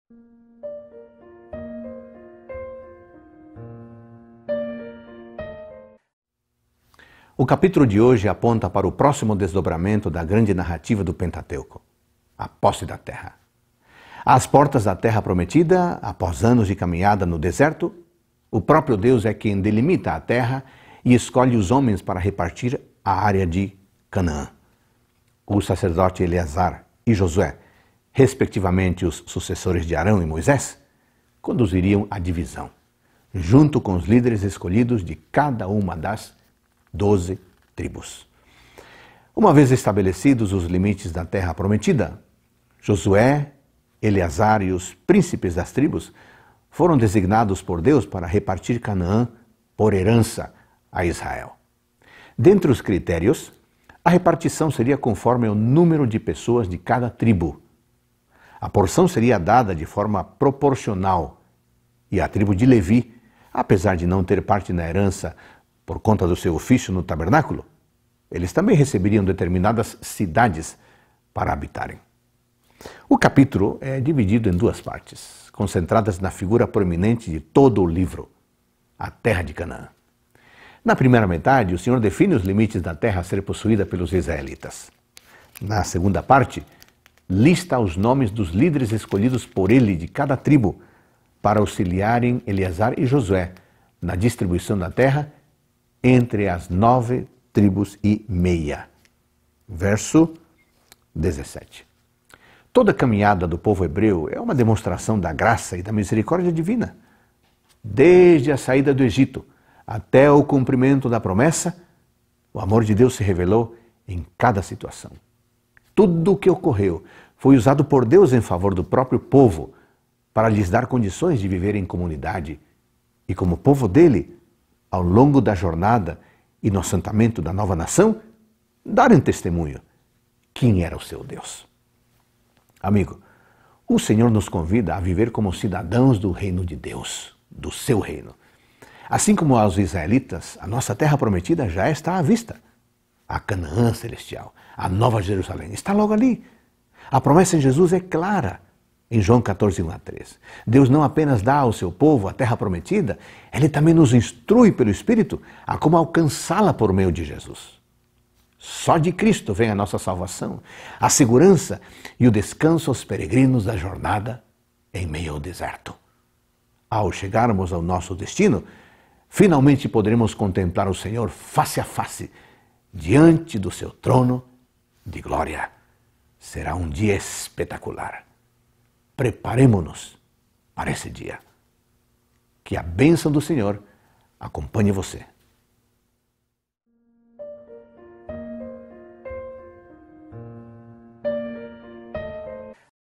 Números 34 - Reavivados por Sua Palavra
Às portas da terra prometida, após anos de caminhada no deserto, o próprio Deus é quem delimita a terra e escolhe os homens para repartir a área de Canaã.